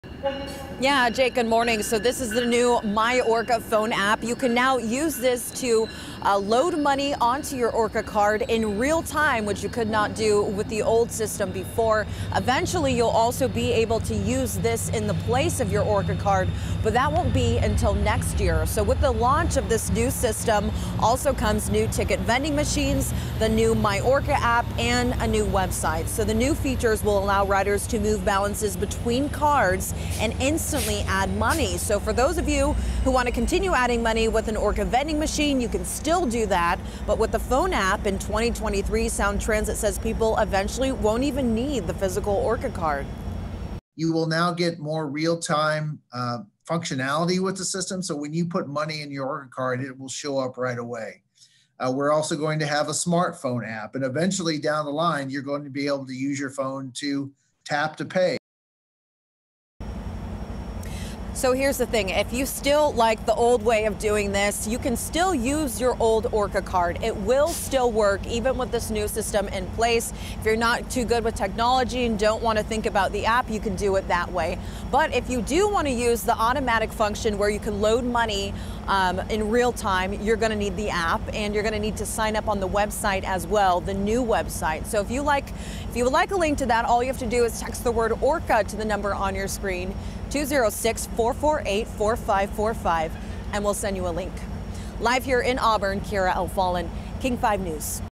New ORCA ticketing system launches
A new ORCA ticketing system launched Monday, aiming to boost ease and efficiency of account management.